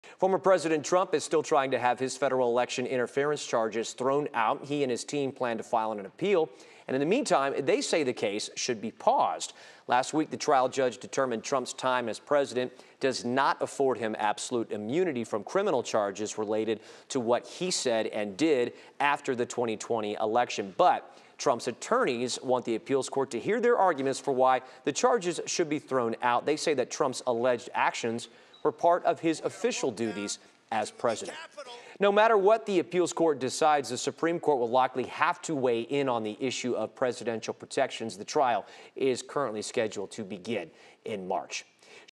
Donald Trump appealing court loss on issue of immunity from federal election interference charges
The trial is set to begin in March.